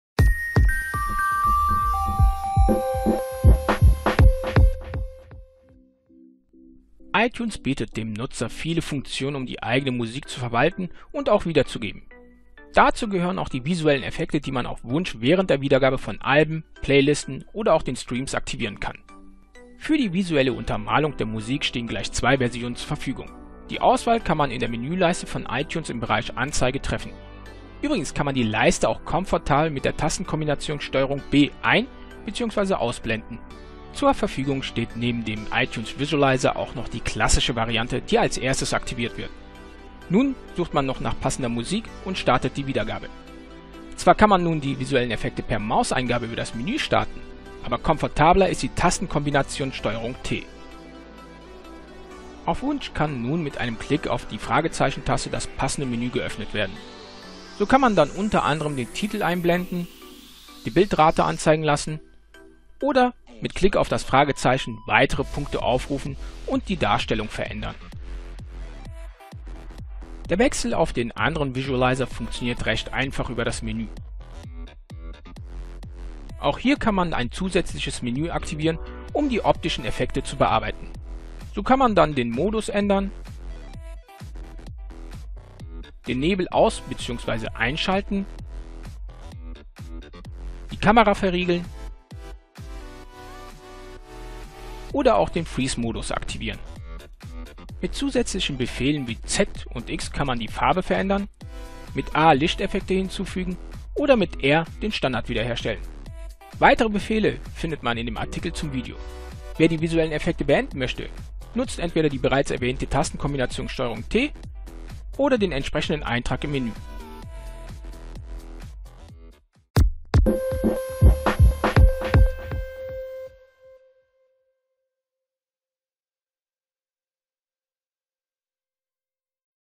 iTunes - Die visuellen Effekte im Überblick | Winload.de
In diesem Video werfen wir einen Blick auf die visuellen Effekte von iTunes. Neben der Aktivierung zeigen wir auch das Menü der Effekte und die zur Verfügung stehenden Funktionen.